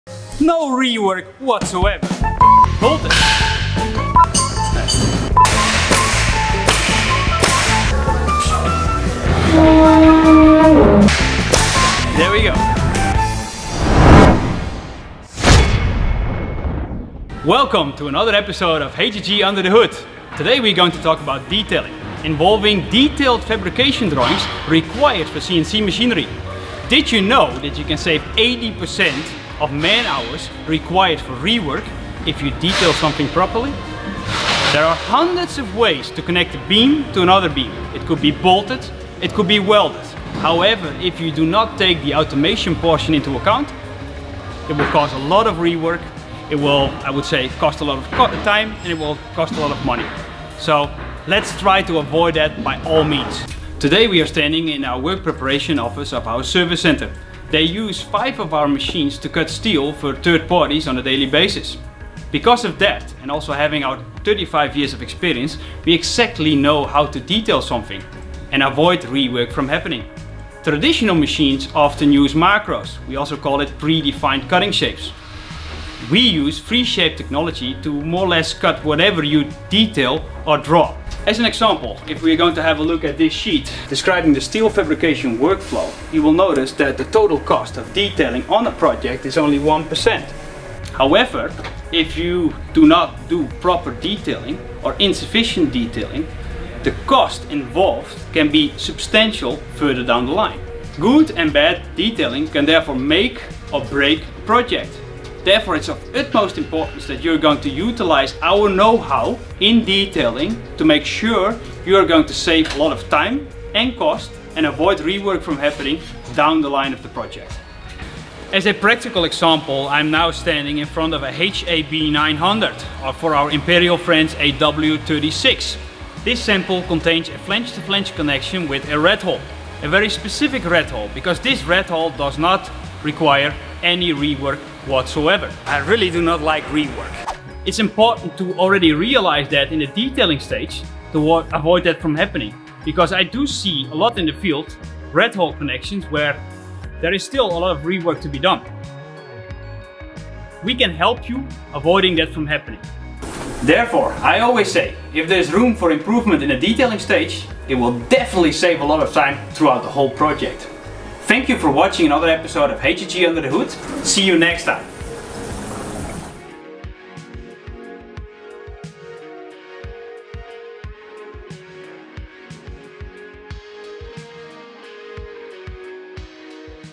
HGG under the hood - Episode 3: Great detailing , no more rework
Today we are going to talk about detailing, involving the creation of detailed drawings for fabricators required for CNC machinery. Did you know that you can save up to 80% of fabricators man hours used for rework, when detailed properly? There are hundreds of ways to in example connect a beam to another beam, either with bolted or welded connections, however if you do not take the automation portion into account, it could cause rework which we obviously try to avoid at all times.

Are you interested to see more technical video's or read knowledge articles?